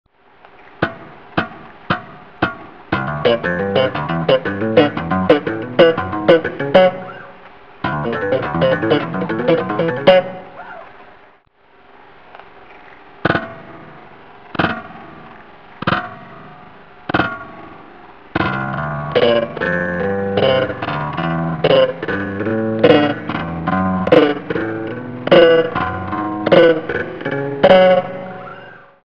Victor Wooten- Open Hammer Pluck
A concept of hitting an open string with your thumb, Hammering a note with your left hand and then plucking a note with your index finger.